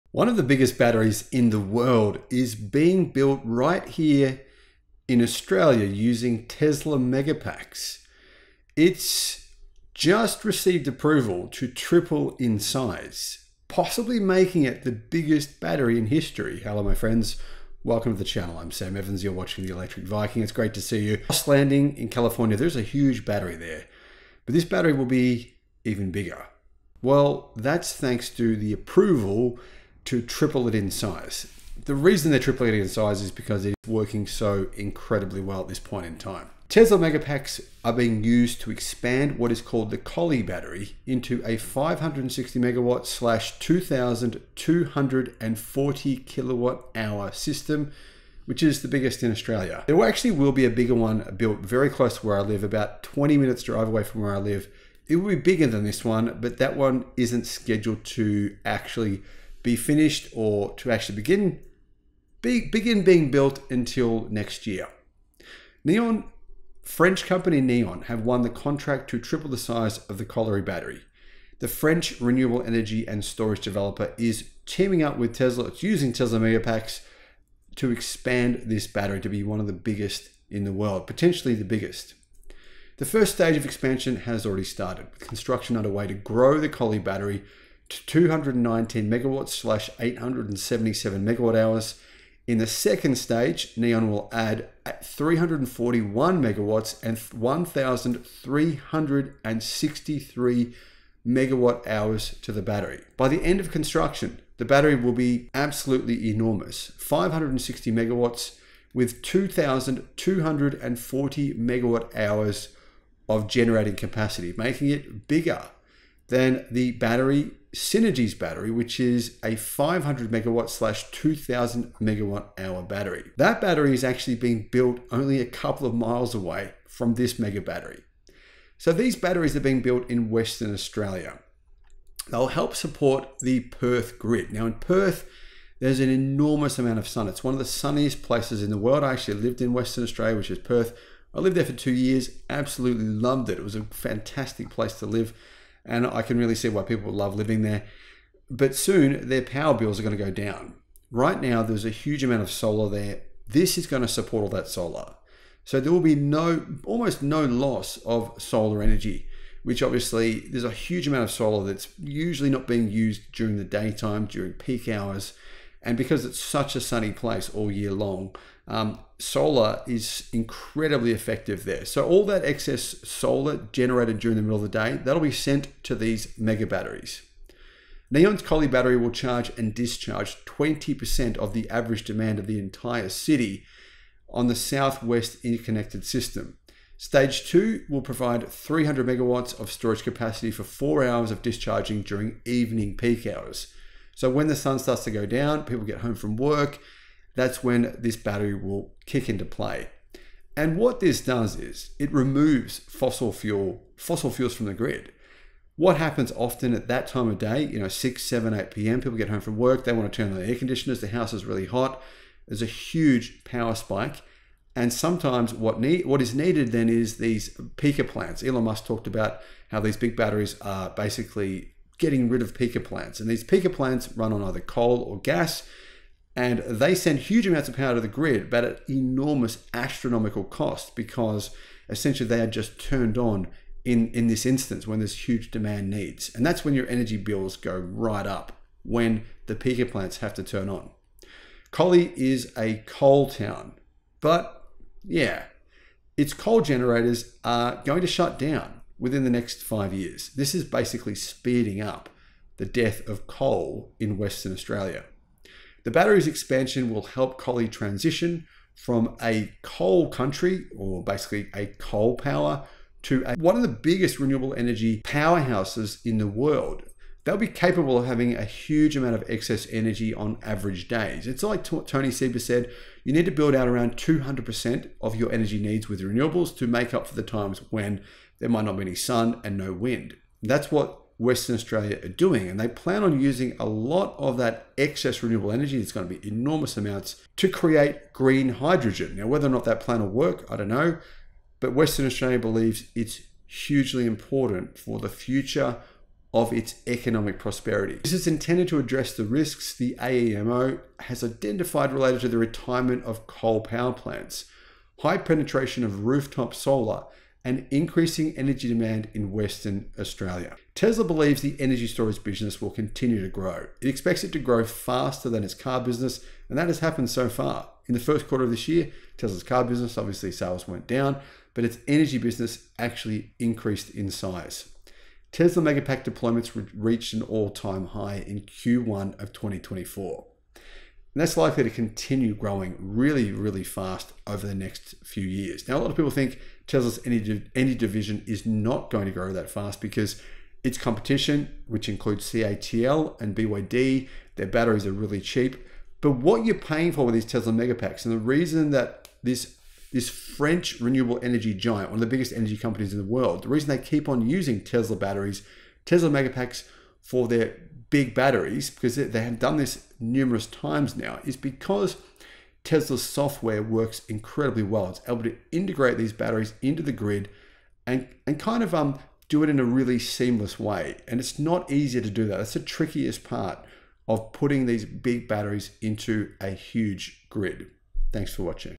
Tesla megapacks deployed to build the biggest battery in Australian history
Tesla slashes the price of the Model Y, Model X and Model S as sales slow